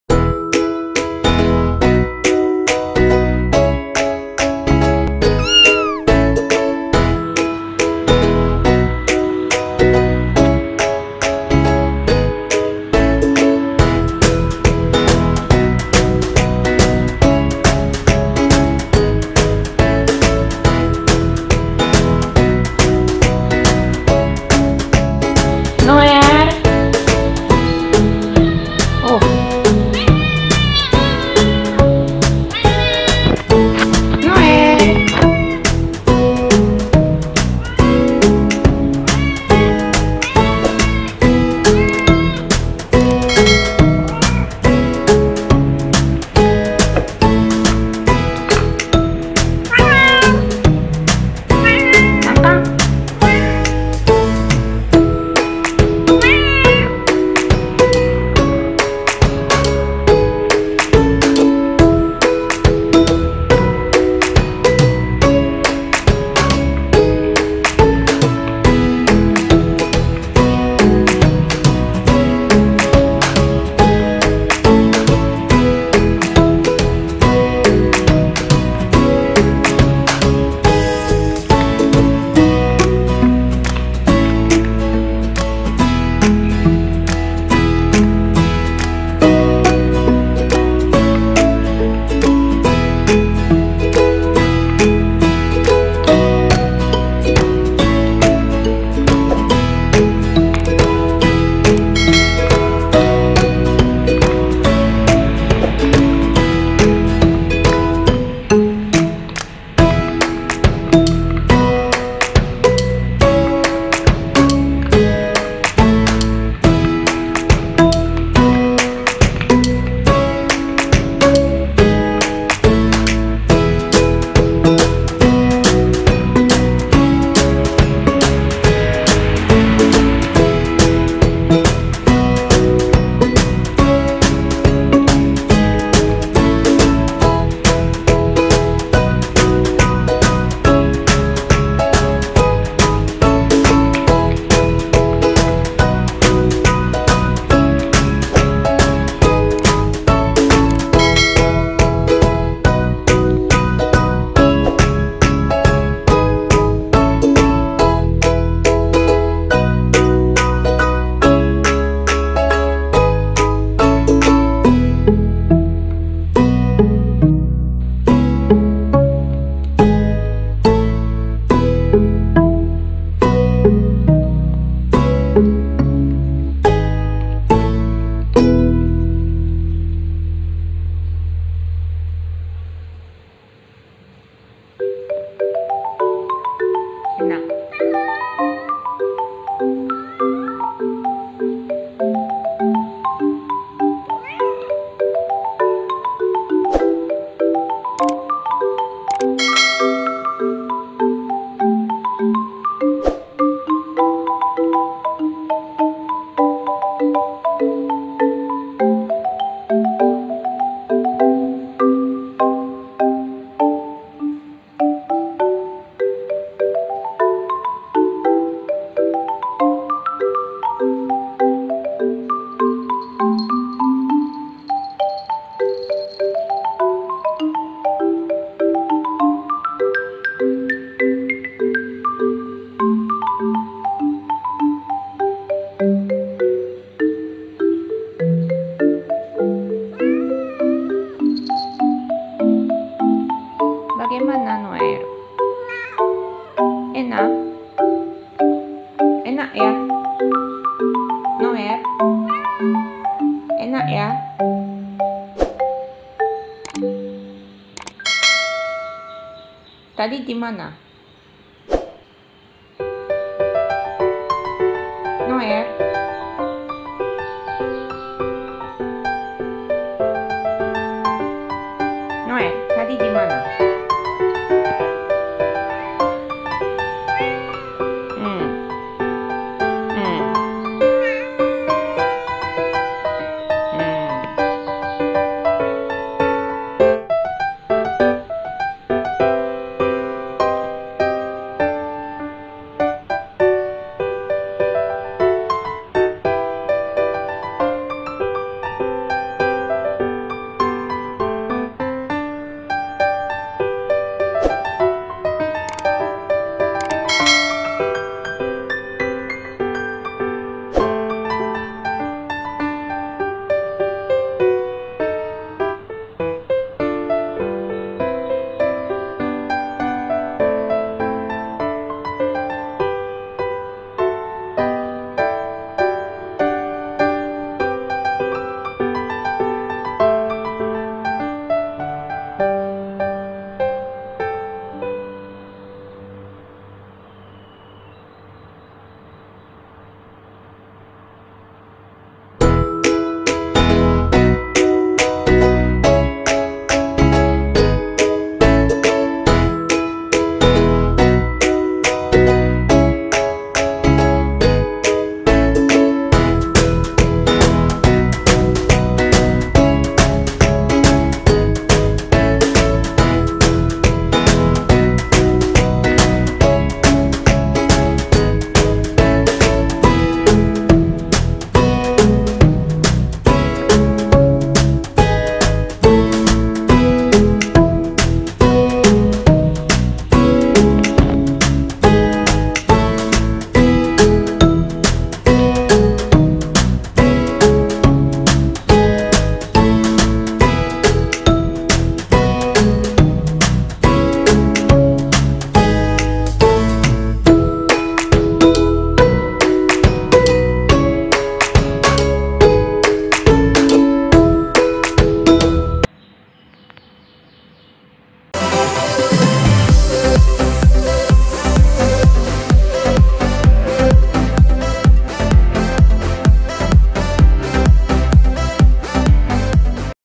Noel The Cat - 6 Month with us and start talking
Noel The Cat - 6 Month with us and start talking kitten cutecat cat ,hi everybody, long time not see, because of these pandemic..but anyway, im already 6 month now and i'm talking

daily activities and activities of our cats, ranging from playing, sleeping, sulking